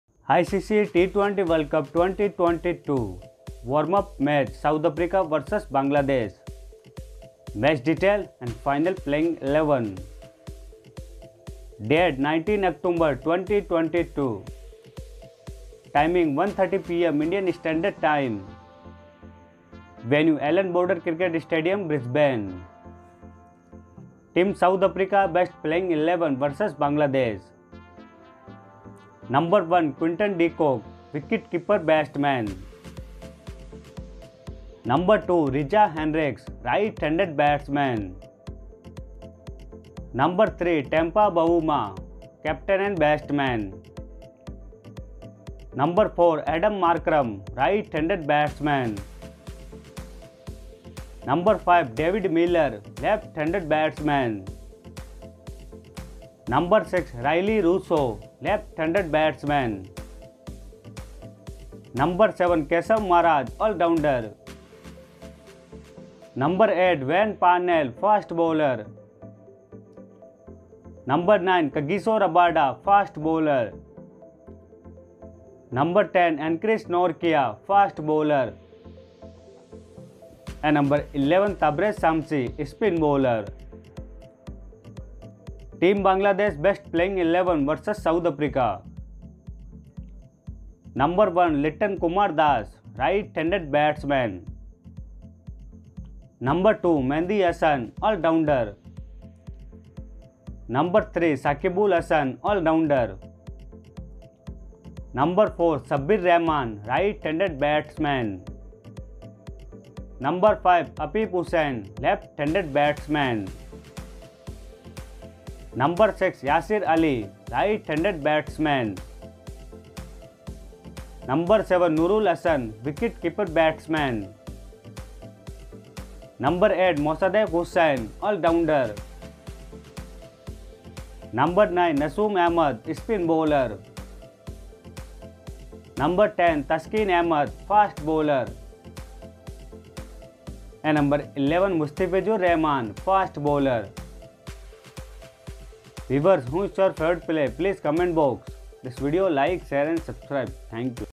ICC T20 World Cup 2022 | Warm-up Match Bangladesh vs South Africa Playing 11|Sa vs Ban T20 World Cup
Match Details

Venue - Brisbane

Bangladesh Playing 11 vs South Africa

Litan kumar Das, Mehdi Hasan, Shakib Al Hasan, Sabbir Rahman, Afif Hossain, Yasir Ali, Nurul Hasan, Mosaddek Hossain, Nasum Ahmed, Taskin Ahmad, Mustifuzur Rahman

South Africa Playing 11 vs Bangladesh

Quendon De Cock, Reeza Henrikes, Temba Bavuma, Aiden Markram, Devid Miller, Rellow Resseow, Keshav Maharaj, Warne Parnell, Kagiso Rabada, Anrich Nortje, Tabraiz Shamsi